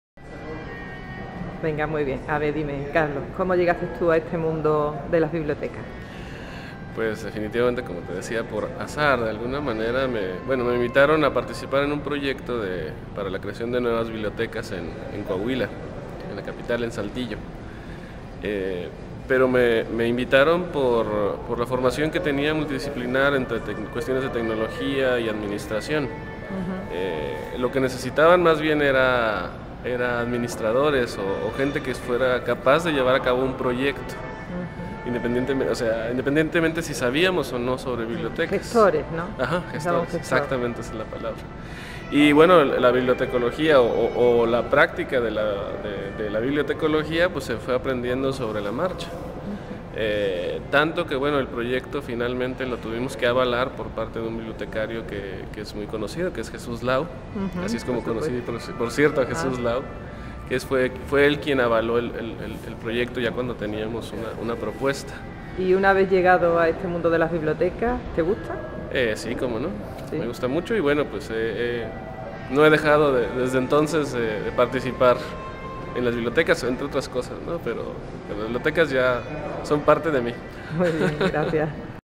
¿Cómo llegué al mundo de las bibliotecas? Carlos E. Montano
Carlos E. Montano es el Director del Servicio de Bibliotecas de la Universidad Autónoma de Ciudad Juarez y presidente de los Encuentros en Desarrollo de Habilidades Onformacionales que organiza la UACJ